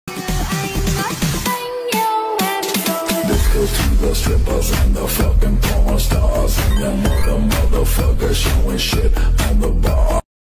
Opal edit ☺️/Main oc/Gacha
Yeah idk, I’m bored and decided to make an edit of my beautiful elf gurl